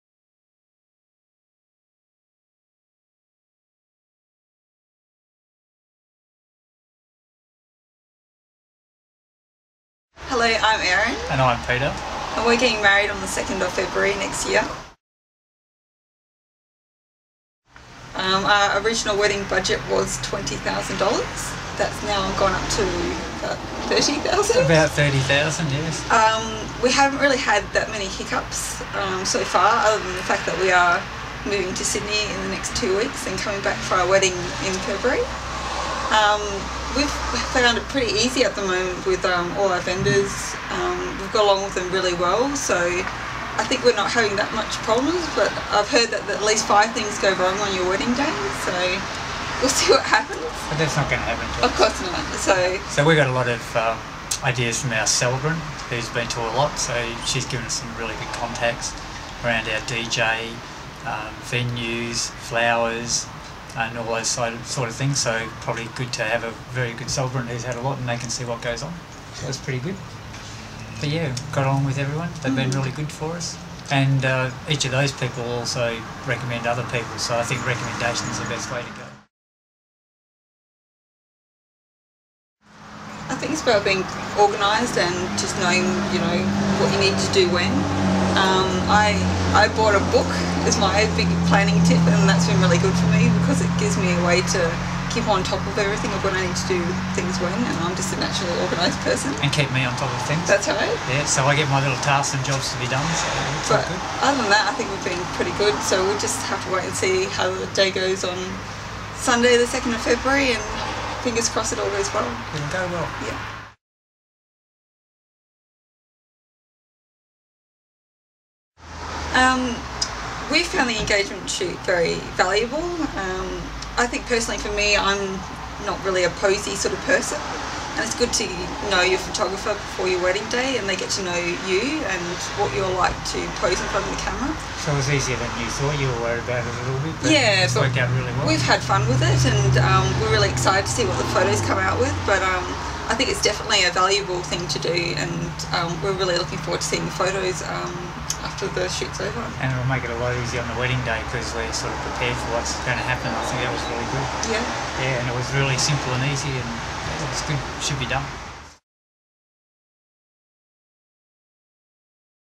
Real Brides - Erin + Peter Engaged
Erin + Peter talk about their wedding plans